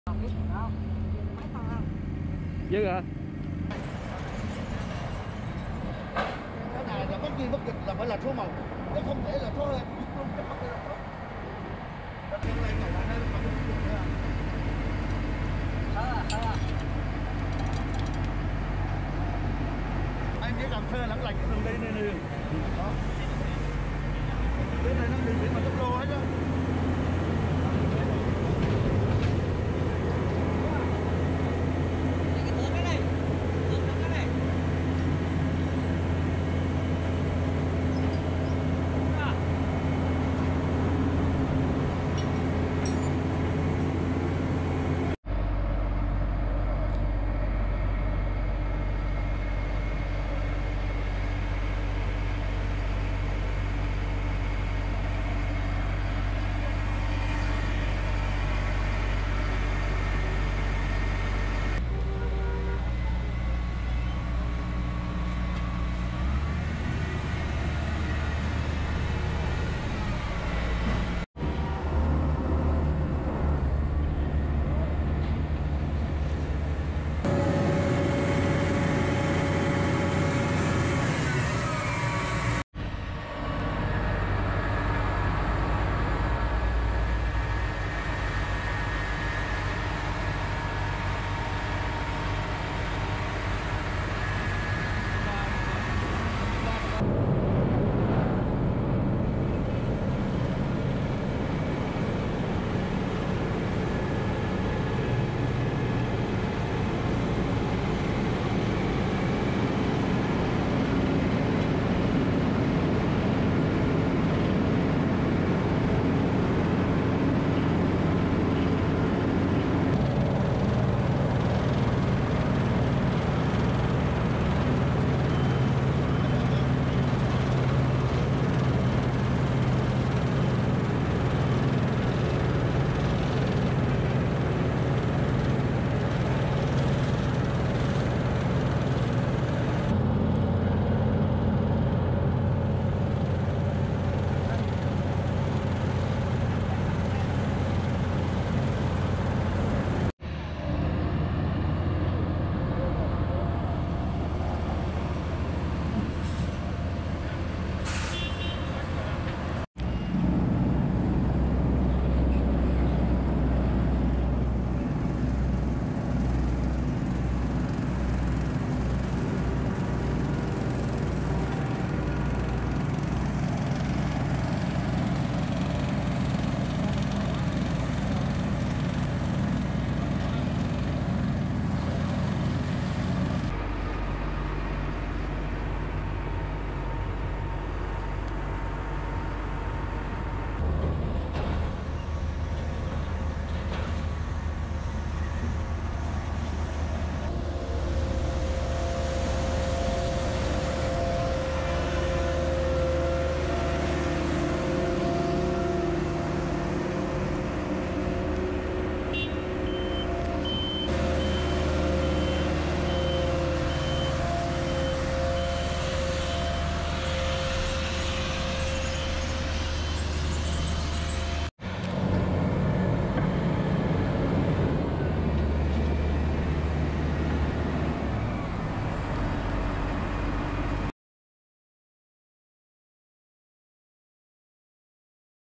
Thảm mặt đường bê tông nhựa nóng
Hình ảnh thi công thử thảm mặt đường bê tông nhựa nóng!